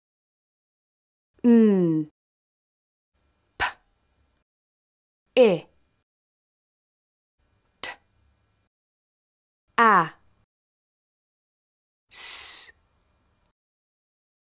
jollyphonics | jolly phonics group 1| satipn sounds | jolly sounds satipn no music
Assalam-o-Alikum,here is phonetic sounds of group 1 jollyphonics without music.